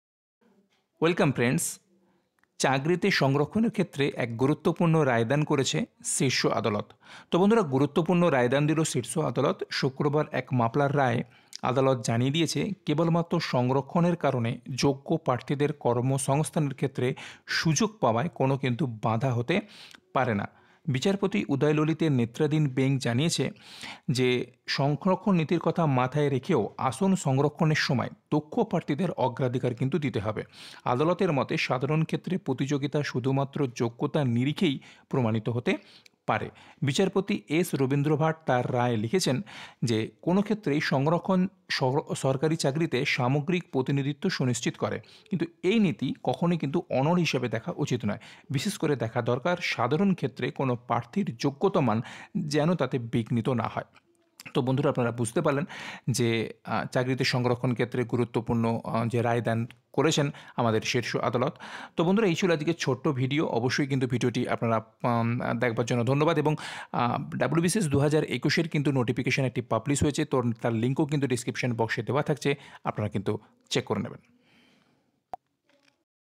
সরকারি চাকরিতে সংরক্ষণ নিয়ে গুরুত্বপূর্ণ রায় দান করল শীর্ষ আদালত
MAA TARATARINI LATEST VIDEO UPDATE NEWS